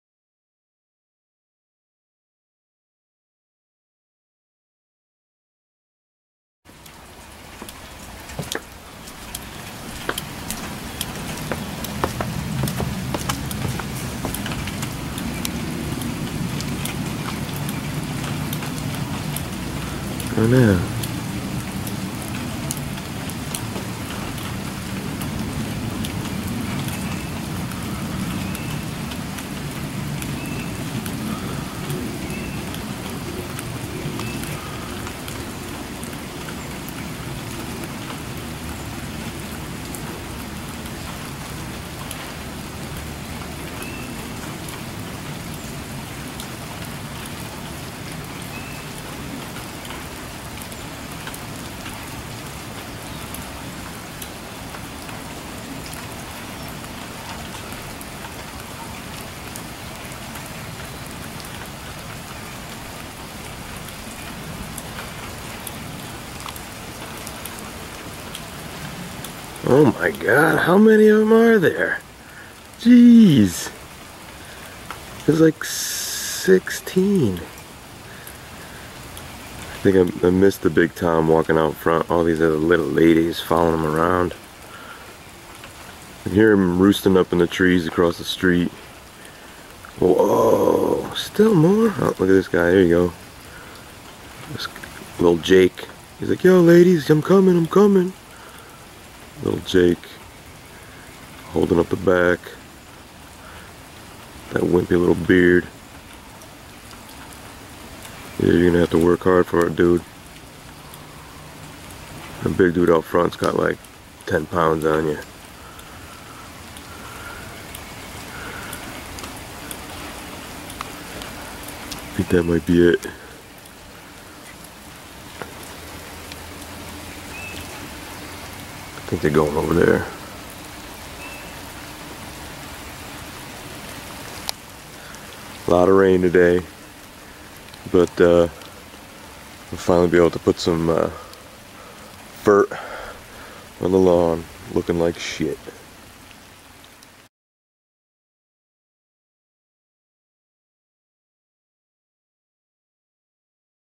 Turkeys In The Sanctuary
Surprised to see them moving around in the rain, it had started to slow down a little and I bet it is the best time to grab some worms.

I don't expect to see this many turkeys in a few weeks, someone in town just the other day got bit by a coyote on the arm... these turks usually will thing out. Tough love in the Sanctuary.